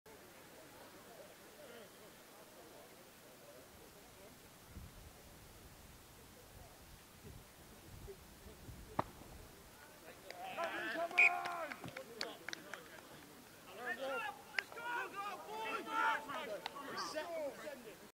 Ventnor v Fawley 1st XV 5/3/22 Clip 12
Fawley win 22-34. Penalty Rob Thomas.